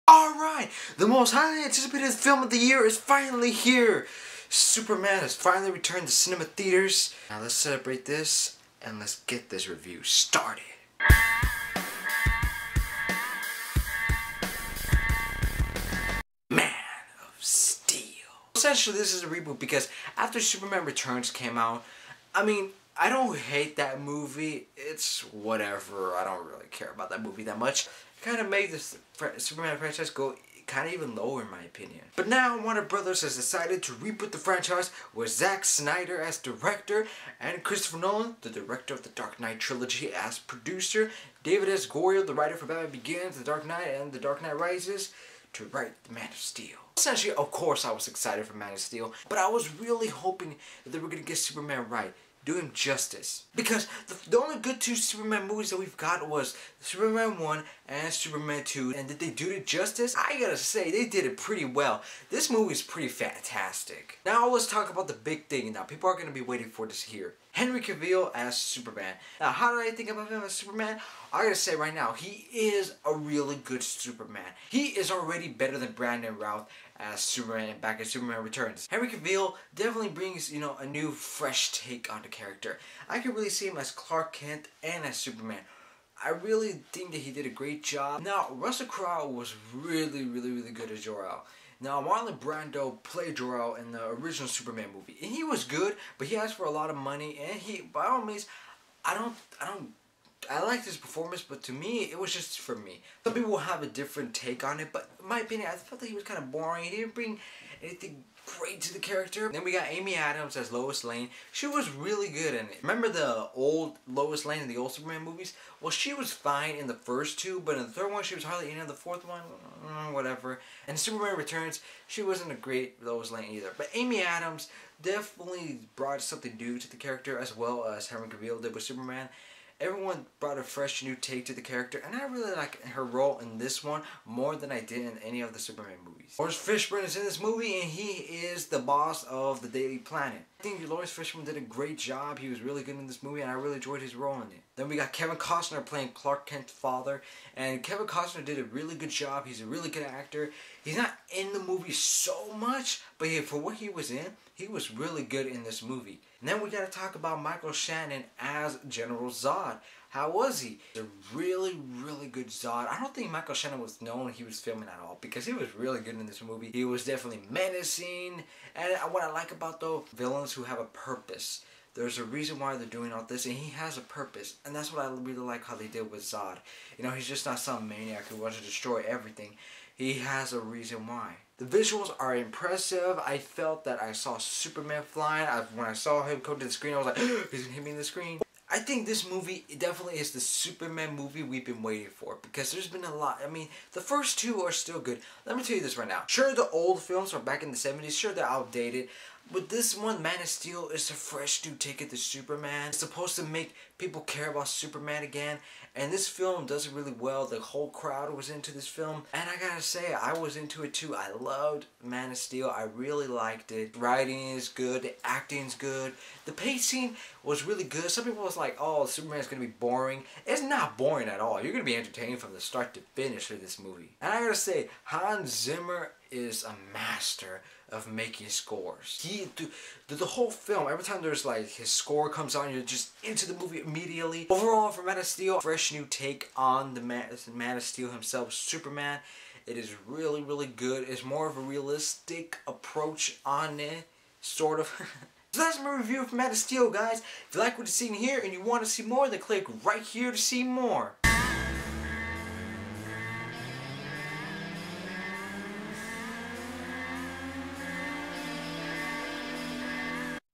Man of Steel Movie Review
Superman gets a reboot that is directed by Zack Snyder (300 & Watchmen), and produced by Christopher Nolan (director of The Dark Knight Trilogy). The most highly anticipated film of the year is here! Alex reviews "Man of Steel", starring Henry Cavil, Amy Adams, Michael Shannon, Russell Crowe, and Laurence Fishburne. Directed By Zack Snyder